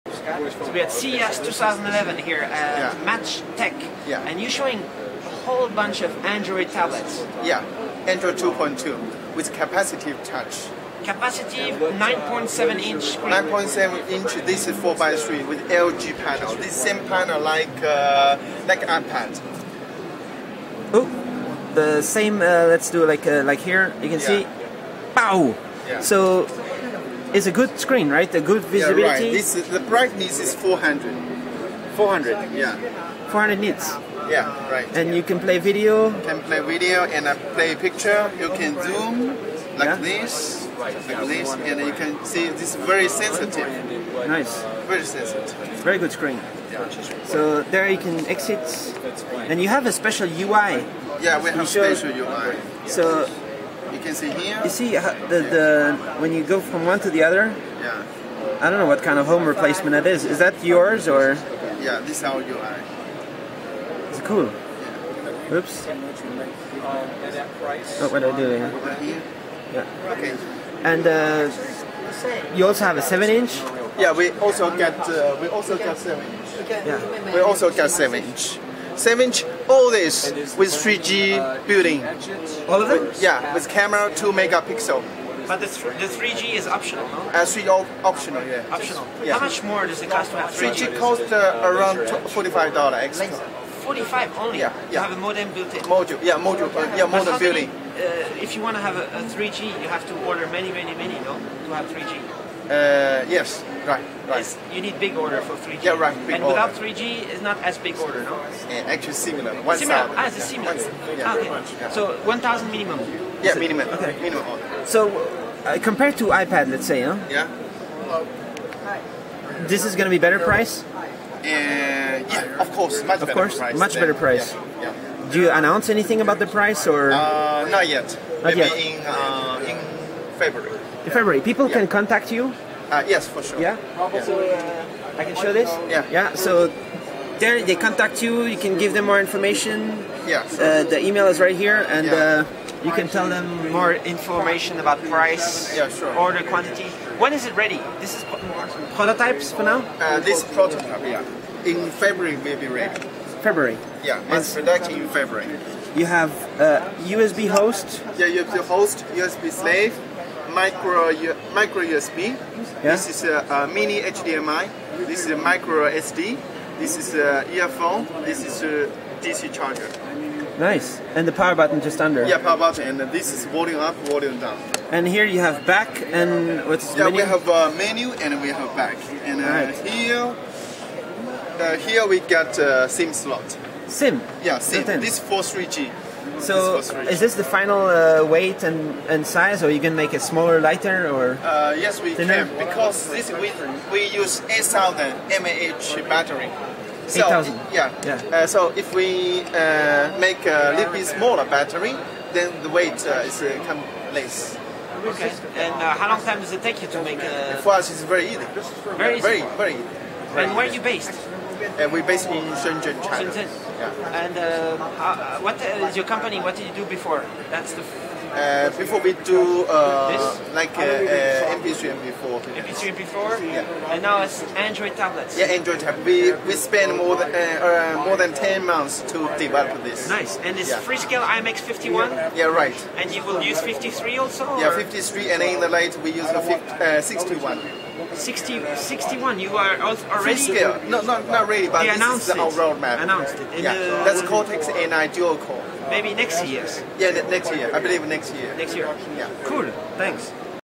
Match Tech 9.7" Capacitive i.MX51 Android Tablet
Here's a similar hardware setup as the iPad, but using the Freescale ARM Cortex-A8 i.MX51 processor, it includes a same 9.7" capacitive LCD panel from LG.